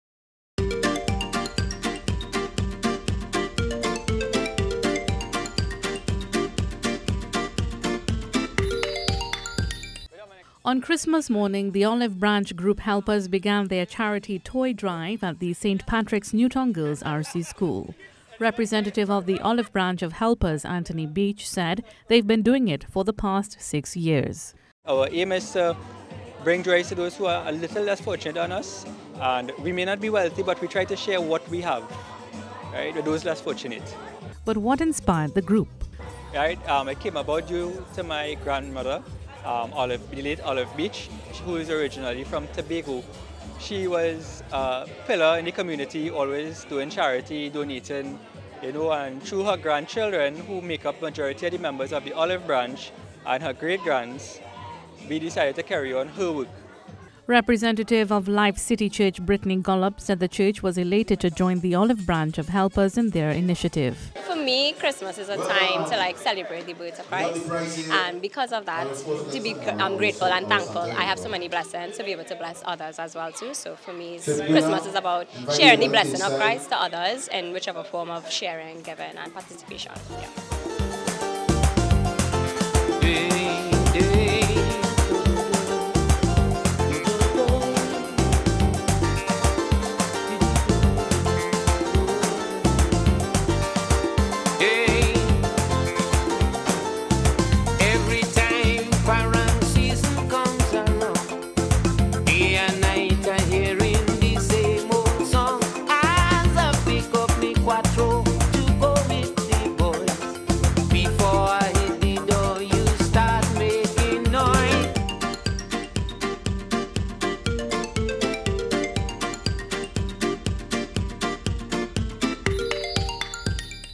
Feel Good Moment: Olive Branch Group Of Helpers
Honoring their grandmother’s legacy is the aim of the Olive Branch Group Of Helpers.

On Christmas Day, the grandchildren of the late Olive Branch gathered for yet another year to assist families in need.

It’s definitely something to Feel Good about!